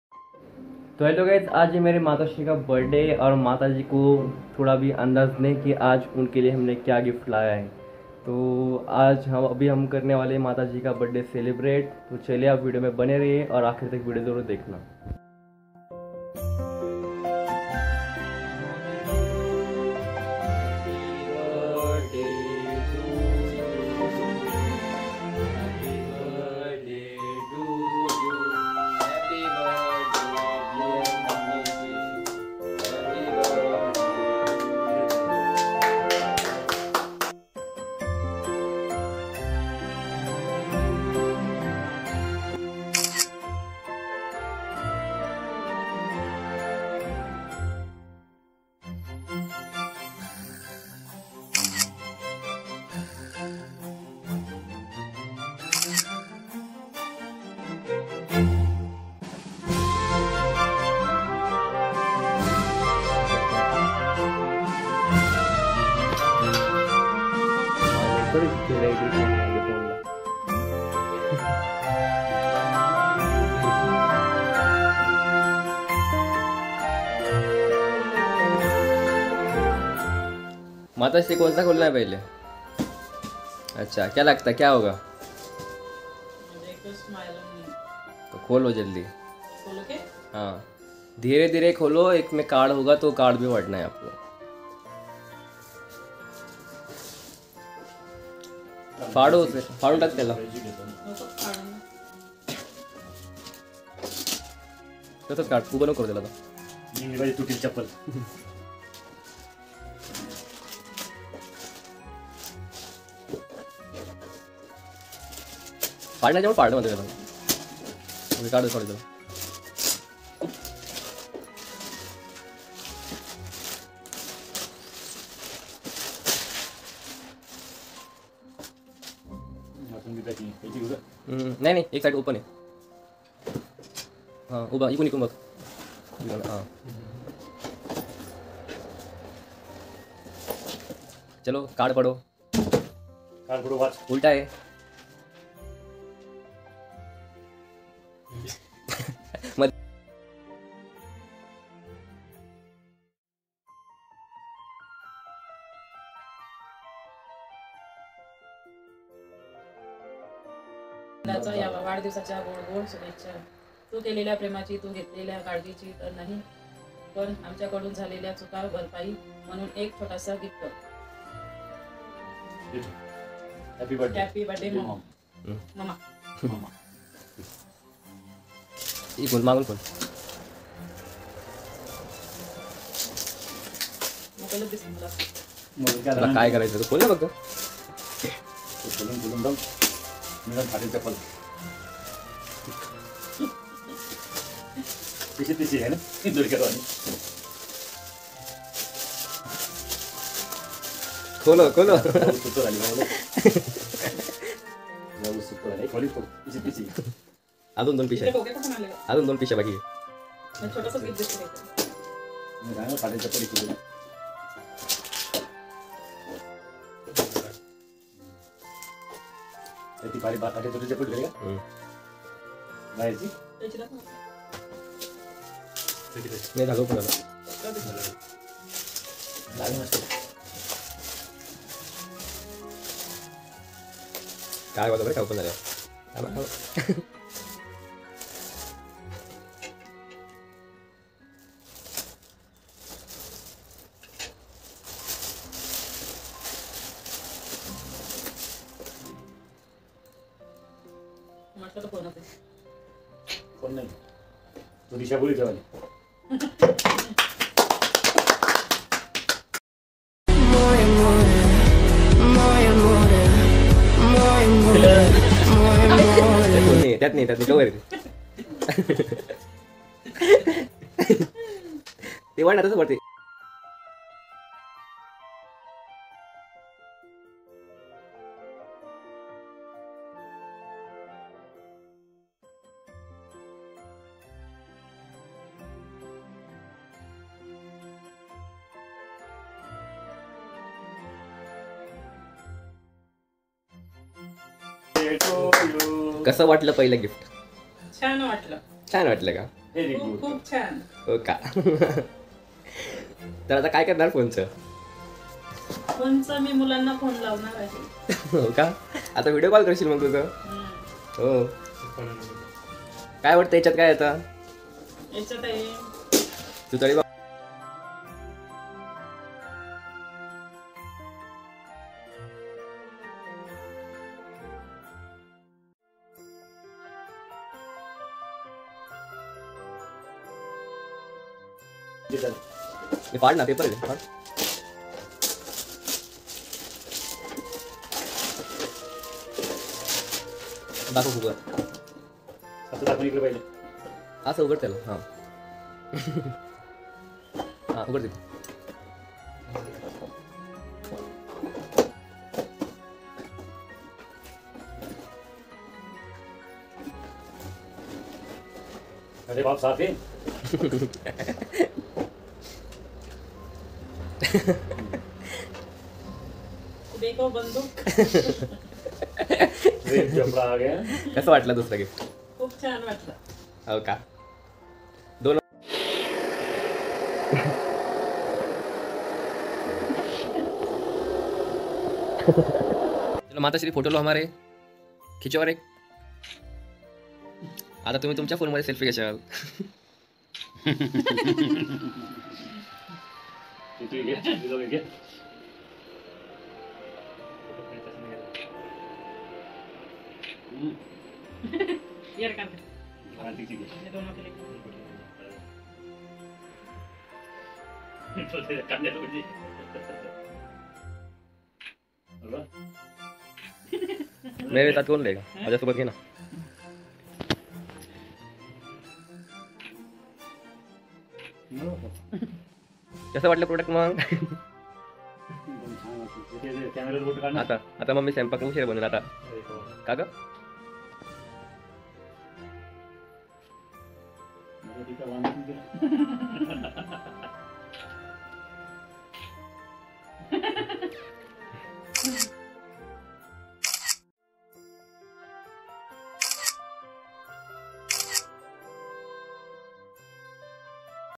Happy Birthday Mom💰 | birthday celebration with opening mystery Box 🎁
ISO :- In this video we are going to celebrate my mother birthday party like of cake cutting and more surprise is goes in opening in mystery box and after opening mystery Box mom is very happy to look her beautiful gift and mission successful passed ❣️ , Happy Birthday to Mummy 🎁🎉